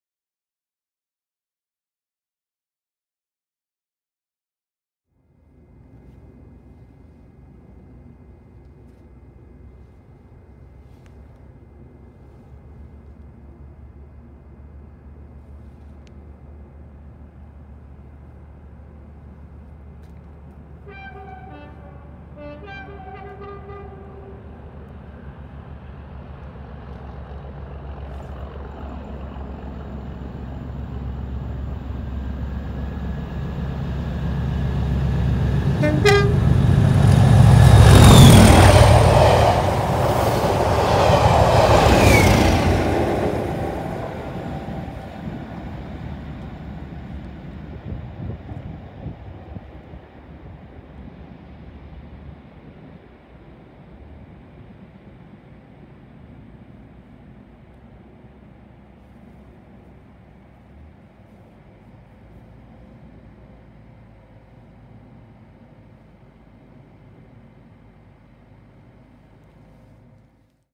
Colas Rail 37610 and 37612 toil past Manningtree with 1Q99
With Our Colas Rail Driving Friend up front, Colas Rail 37610 and 37612 toil past Manningtree with 1Q99 from Cambridge to March.